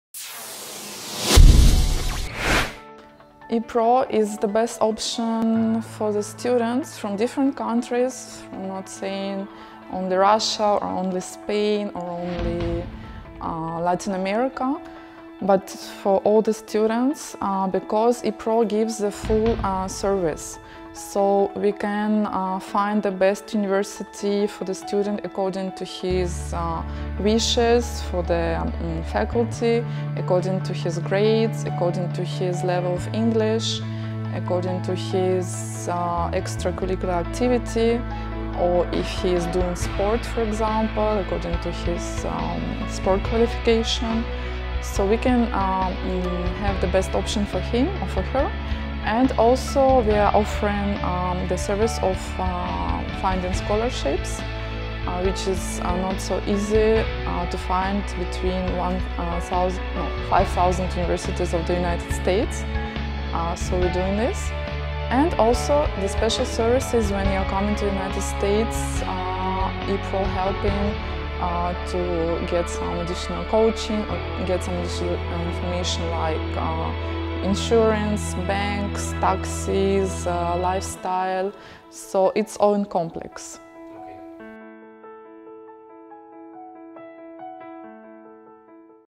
Why Epro 360 is the best option for students
Dreaming about studying in the U.S.? We can help you.

Epro 360 is an organization that offers professional assistance to study at any institution among the 5,000+ that exist today in the United States. We provide services for both undergraduate and graduate international students and help them find scholarships to considerably reduce their cost to study in the U.S. Our team is formed by former international students with a passion to help the future generation of international students get the most out of their experience studying abroad.

Hundreds of students from around the world have already benefited from studying in the U.S. with the help of Epro 360.